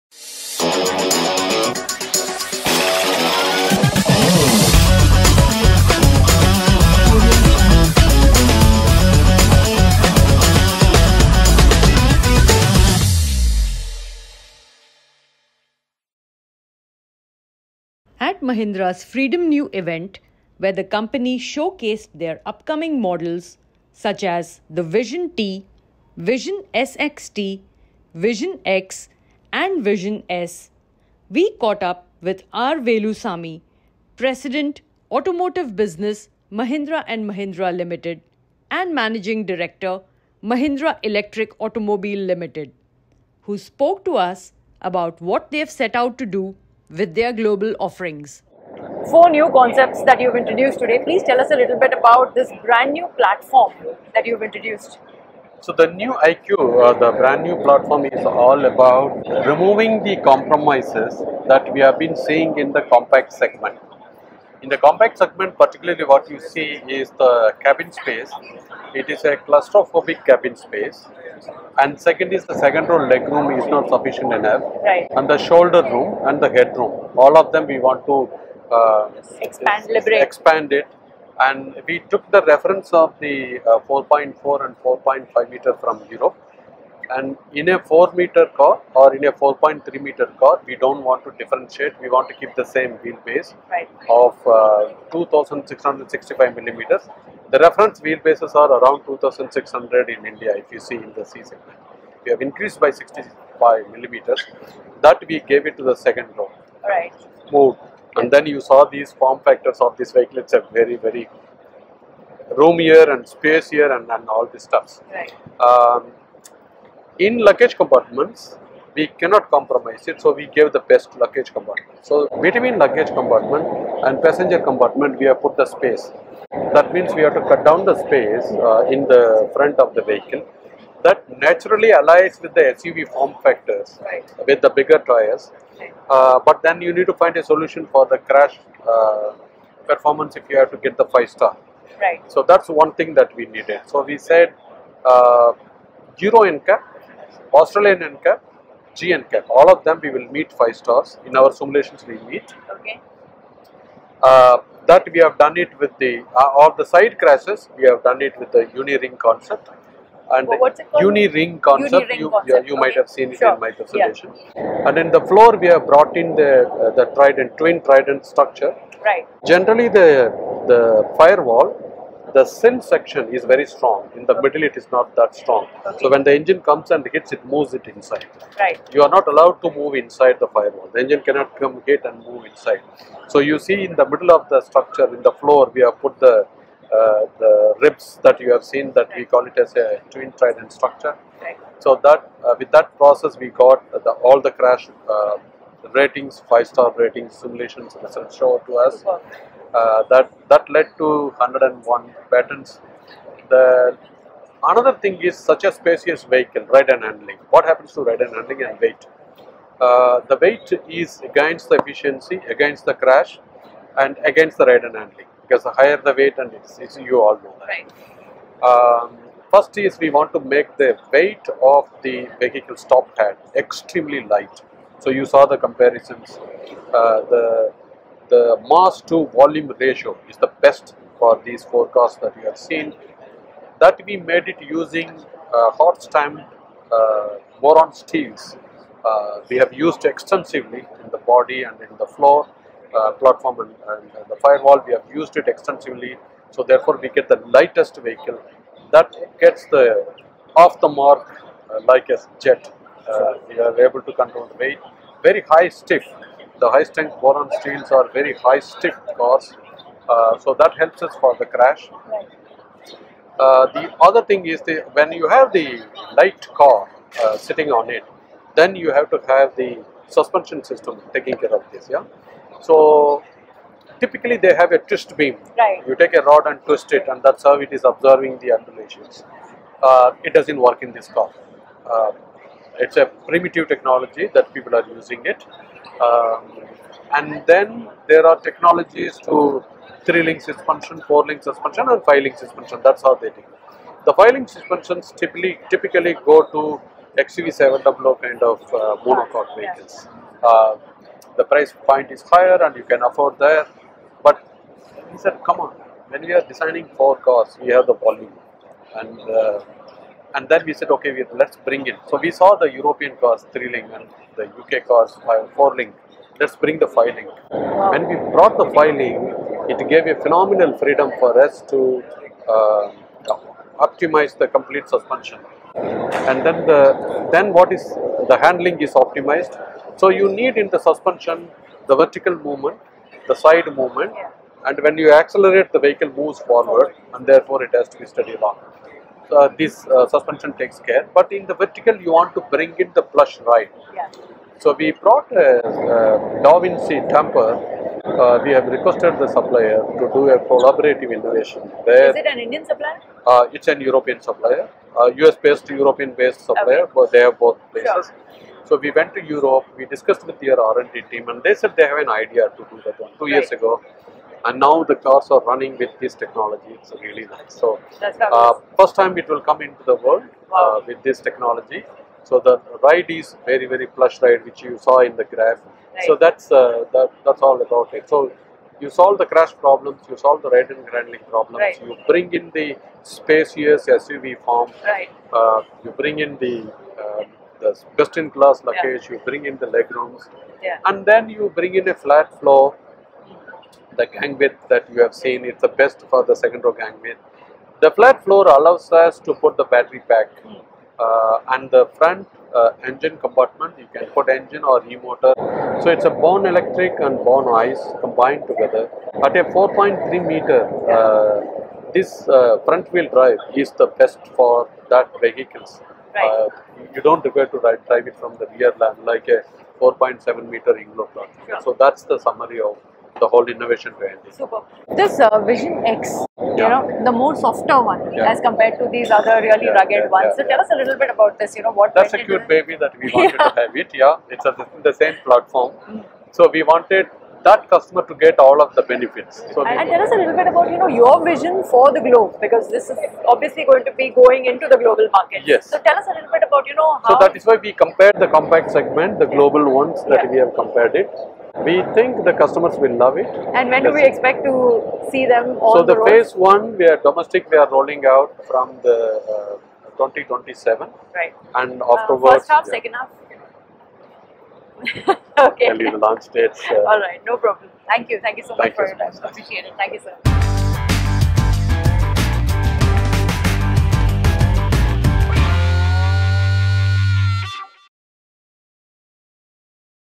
Talking Technology with R VELUSAMY of Mahindra & Mahindra Ltd.
In this video, R Velusamy, President - Automotive Business (Designate), Mahindra & Mahindra Ltd., speaks with Rachna Tyagi, Editor, TURN OF SPEED, about the company's new platform nu_IQ and all the latest technologies that they are bringing on their new models which will be coming not just to India but also to the global markets in 2027.